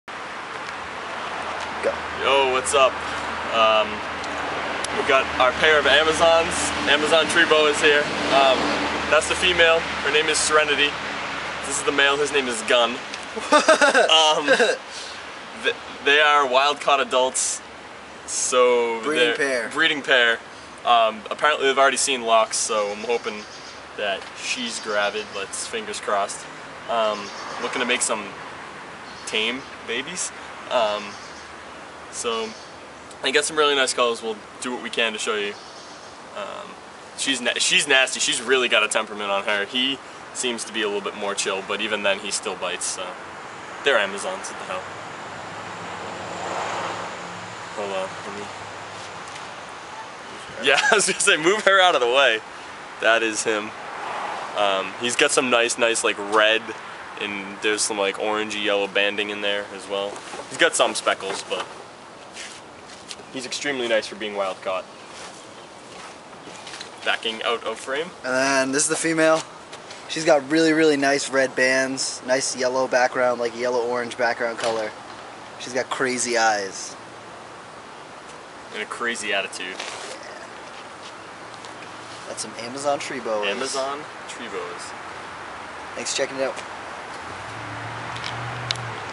Amazon Tree Boas!!
This is my pair of Amazon Tree Boas (Corallus Hortulanus) They are a wild caught breeding pair, with temperaments to match ;) I am holding the male which is the smaller and redder or the two. Rob is holding the female who is larger and she is more orange and yellow. Please feel free to comment rate subscribe. special thanks to my friend Rob you can check his channel out and please if you like amazon tree boas check out get and amazon and join up! its the right thing to do!